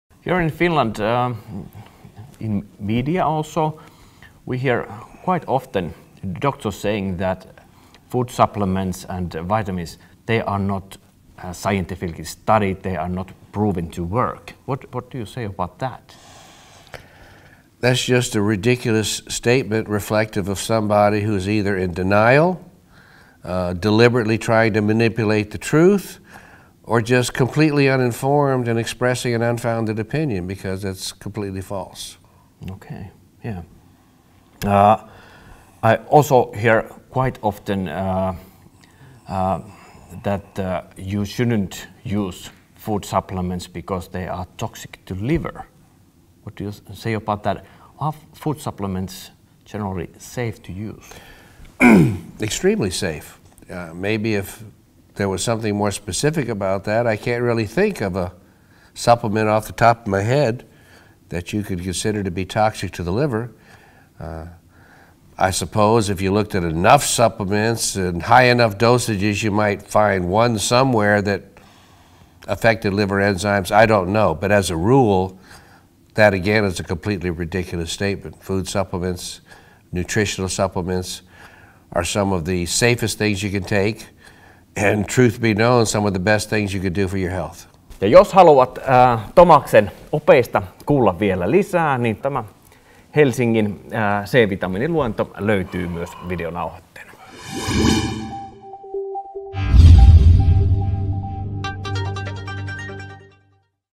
Onko C-vitamiinia tutkittu tieteellisesti?
C-vitamiinin asiantuntija, lääkäri Dr. Thomas Levy vastaa väitteisiin, että vitamiineja ja muita ravintolisiä ei olisi tieteellisesti tutkittu ja niiden ei olisi todettu toimivan. Näet koko haastattelun ja 8 tuntia luentovideoita hankkimalla katseluoikeuden Dr. Thomas Levy Suomessa 2020 -luentovideoihin osoitteessa